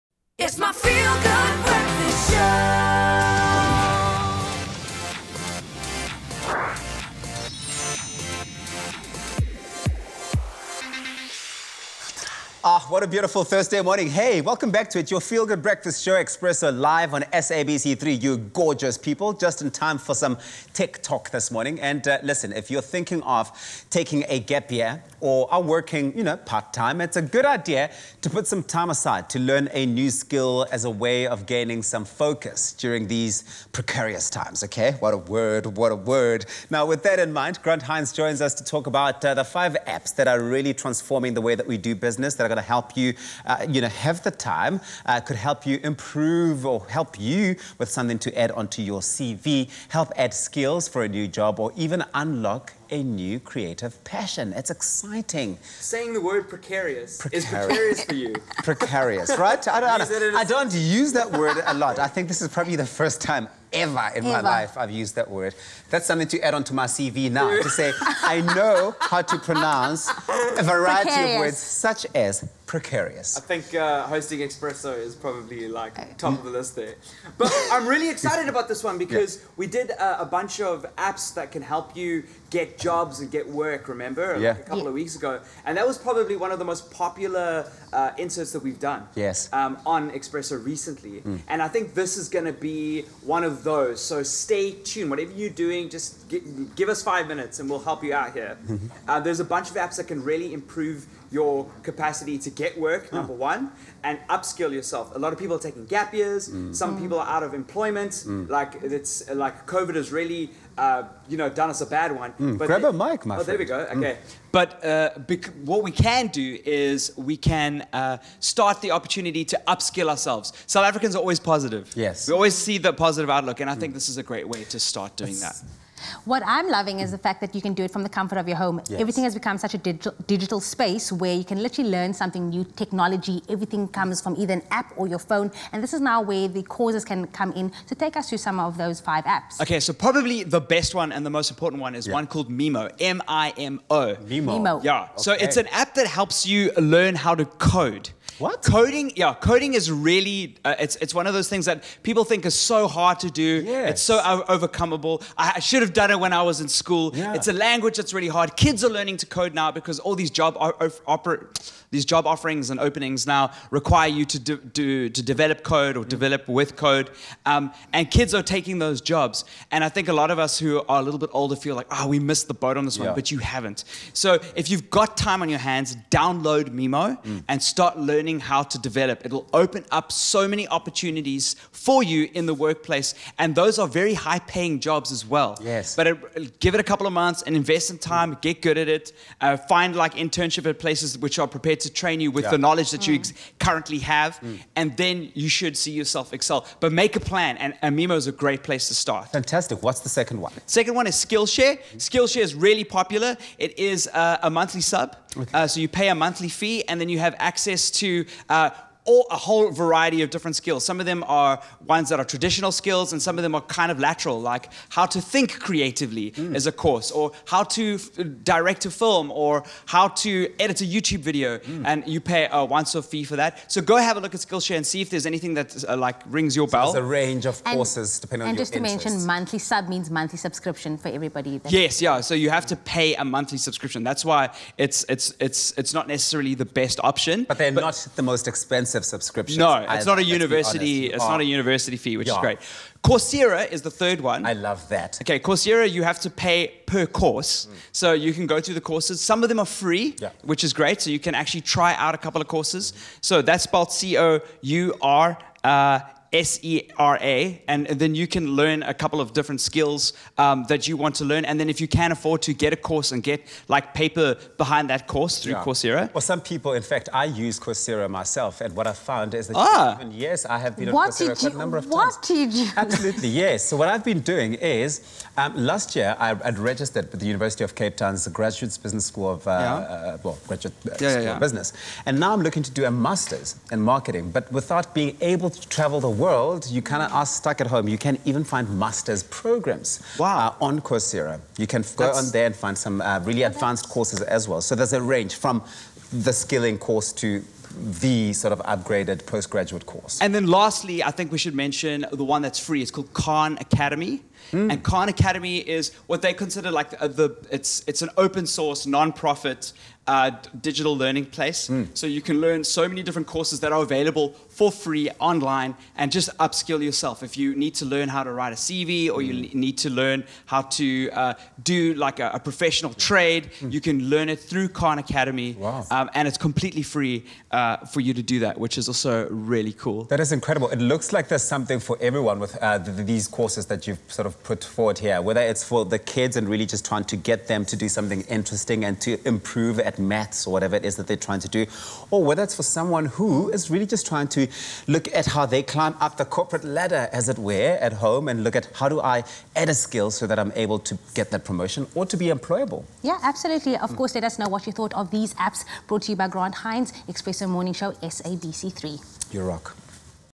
Tech Talk: 5 Apps to Help you Upskill
From learning a new language to learning a computer one for coding; there’s an app for that.